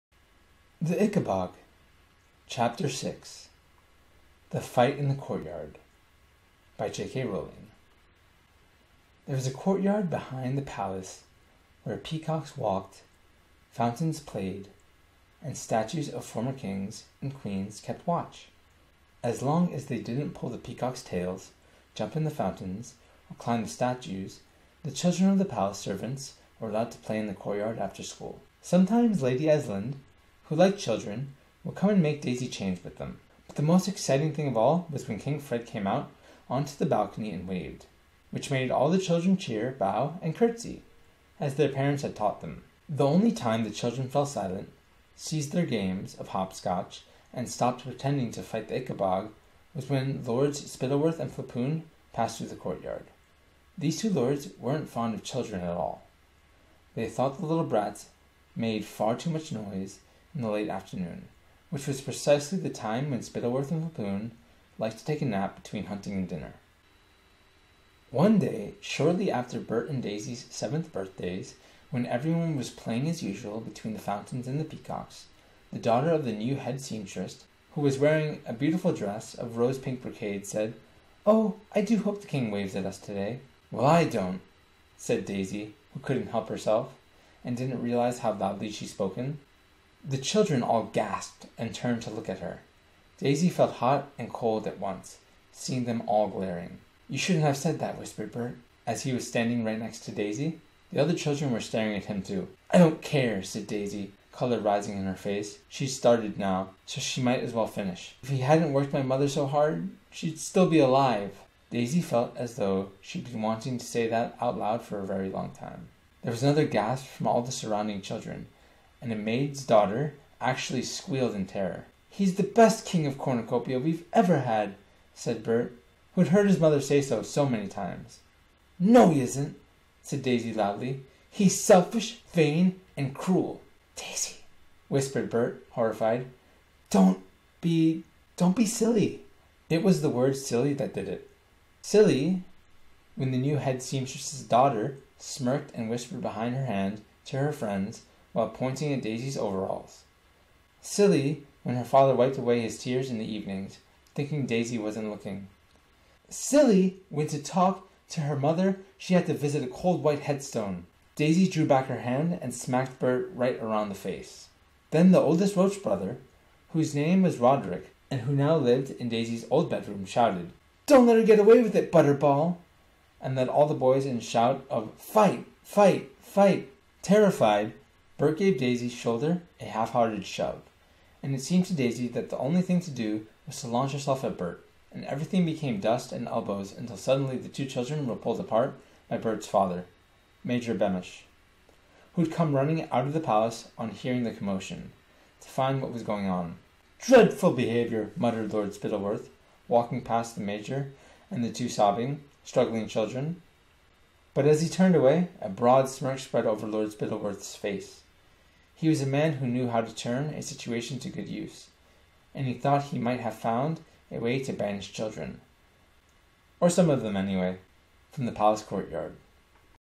The Ickabog Chapter 6: The Fight in the Courtyard
JK Rowling released a new book online for kids, I thought I would read it for you all :)